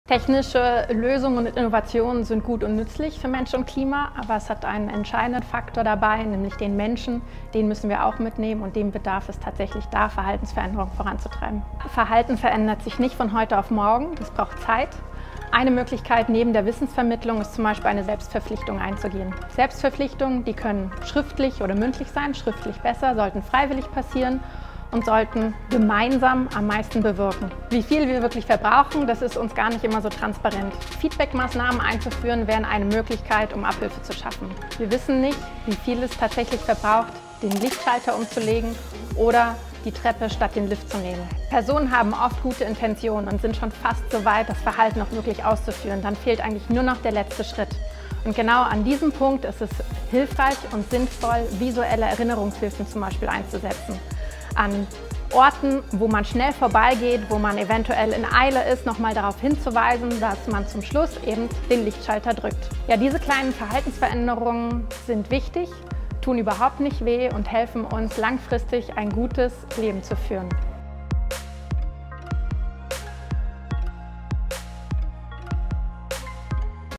Verhalten beim Energiesparen
Warum der Mensch beim Energiesparen ein entscheidender Faktor spielt, erläutert Umweltpsychologin Cathérine Hartmann vom ZHAW-Departement Angewandte Psychologie.